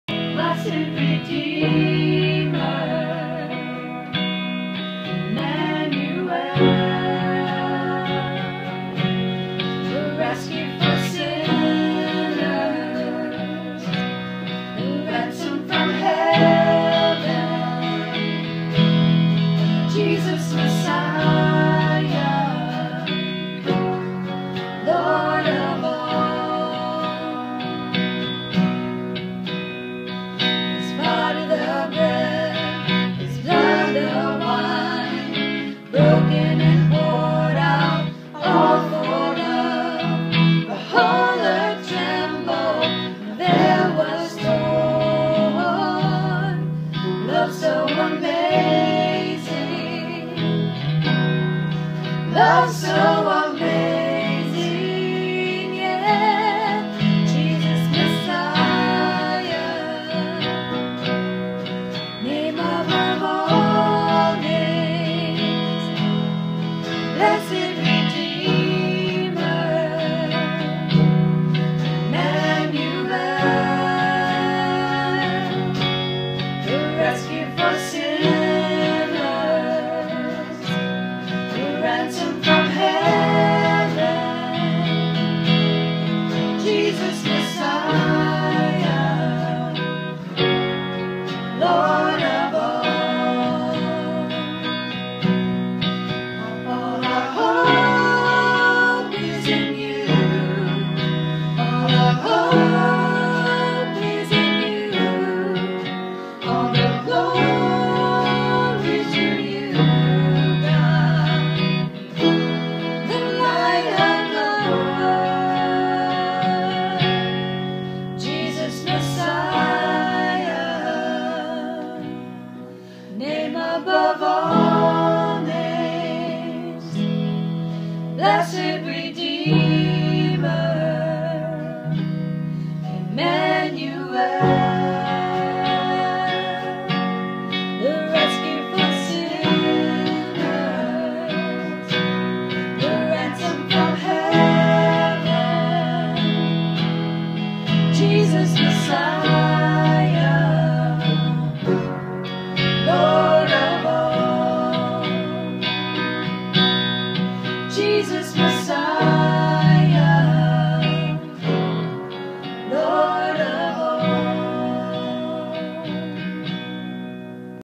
Common Ground - Worship Team Rehearsal 3-30-2013
Tim Sherman, Allison Audoire, Susan & Robby Lapp rehearse at 415 S. Palmetto Ave. Sanford, Florida for Common Ground Christian Fellowship.